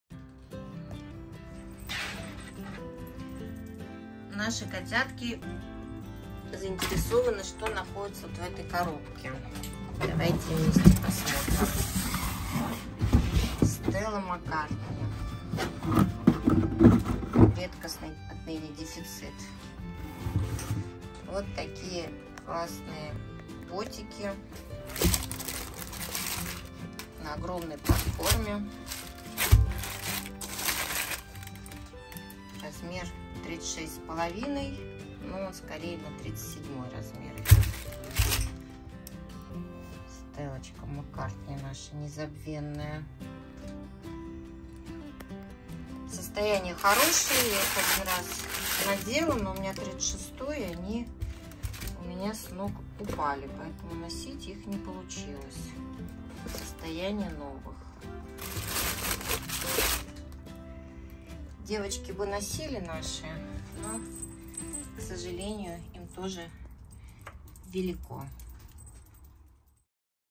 Лоферы Stella McCartney 25,5 см по стельке
Актуально, как никогда. Я эту модель дважды заказывала на Yoxx,  когда он ещё работал в России, и оба раза промахнулась с размером. Первый раз заказала 36 - впритык. Второй раз 36,5 - велики.  По стельке - 25,5 см, даже 26 - если впритык. Коробка, чехол в наличии. Север Москвы. Пишите в комментариях, либо в WA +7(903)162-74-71. Мошенников пробиваю сразу, так что не тратье своё и мое время